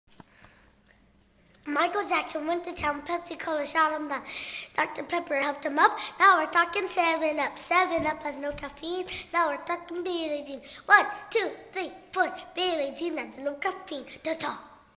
Parker Pannell as a 6 year old Youtuber
6 year old as has rhyme about Michael Jackson.Created on November 21, 2009 using FlipShare.